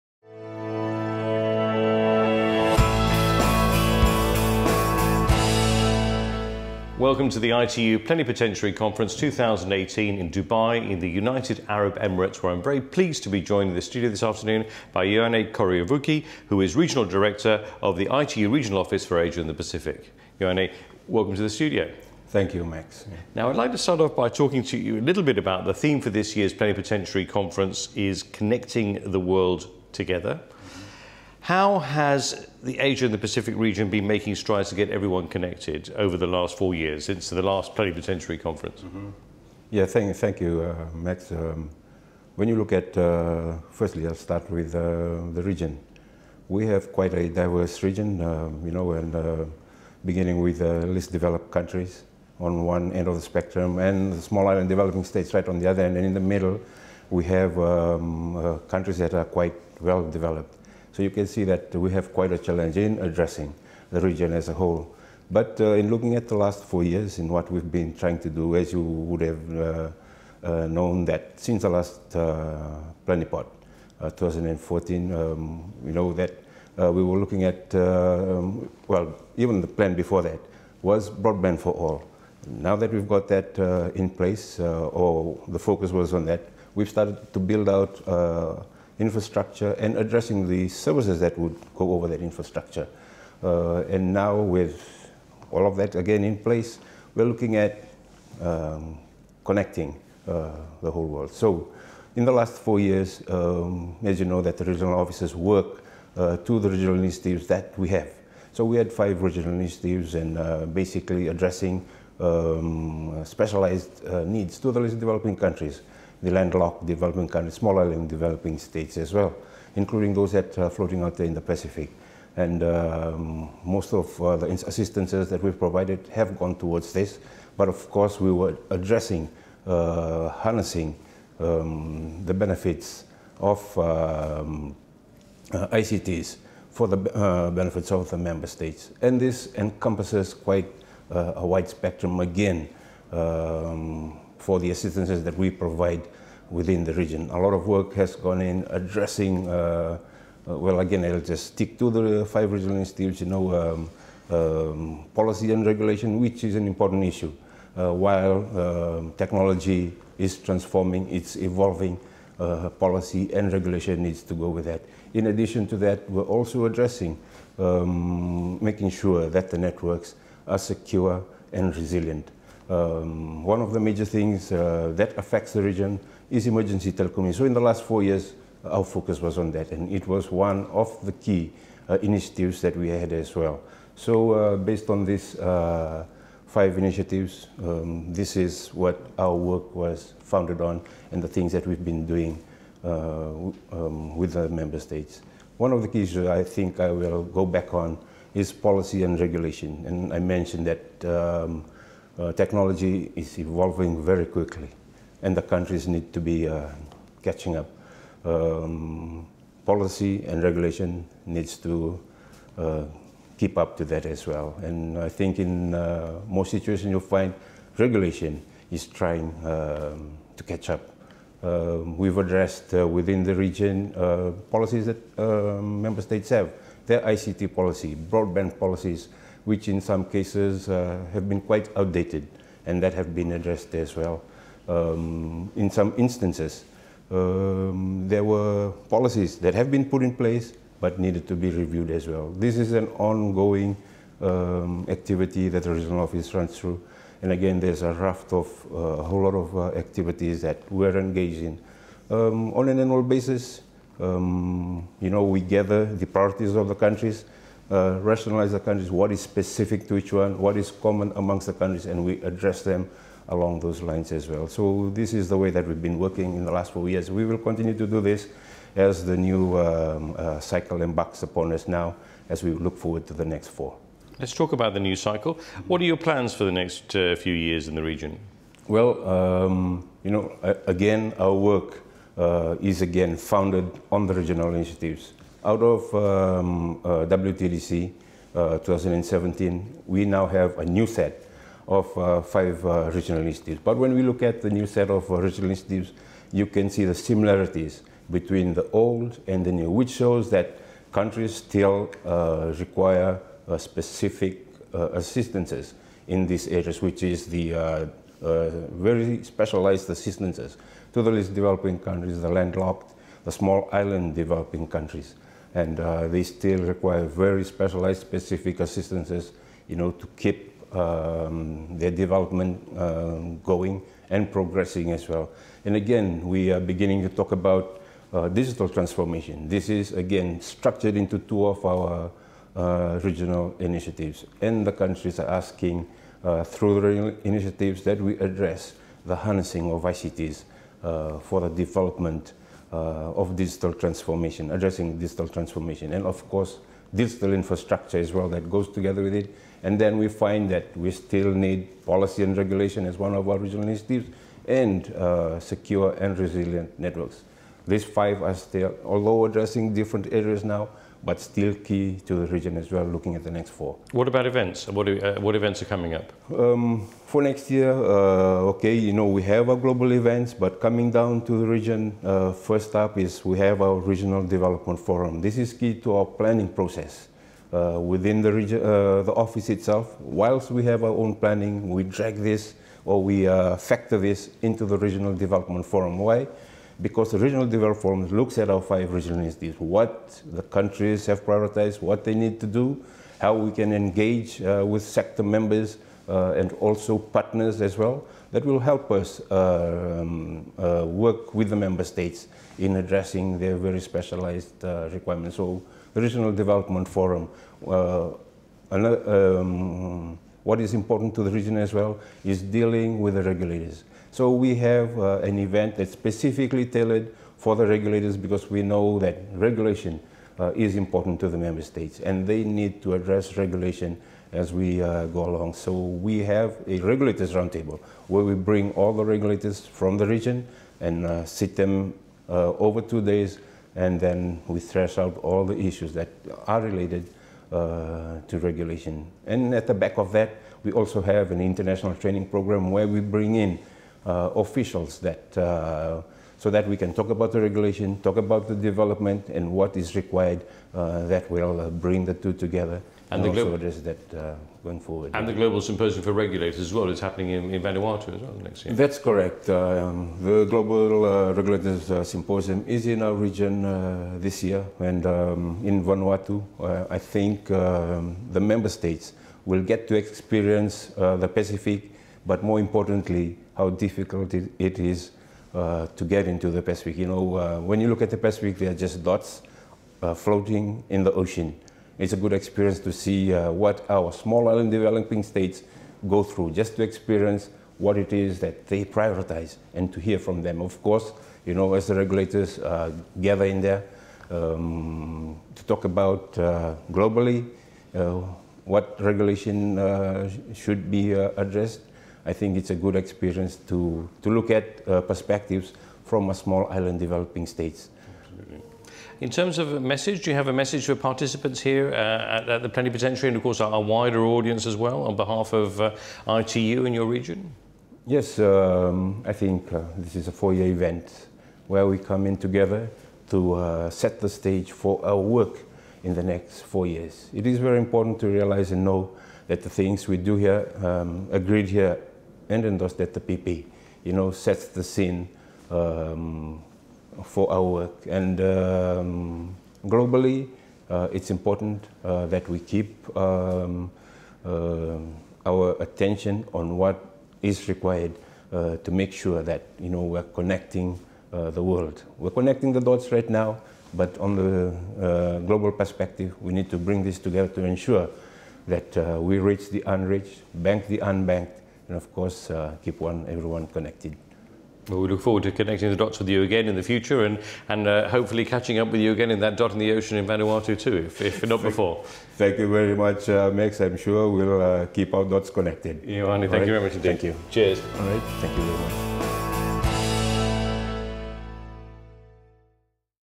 ITU INTERVIEWS @ PP-18: Ioane Koroivuki, Regional Director, ITU Regional Office for Asia & Pacific
Interview with Ioane Koroivuki, Regional Director, ITU Regional Office for Asia and the Pacific, at the ITU Plenipotentiary Conference (PP-18), Dubai, United Arab Emirates, 29 October - 16 November 2018.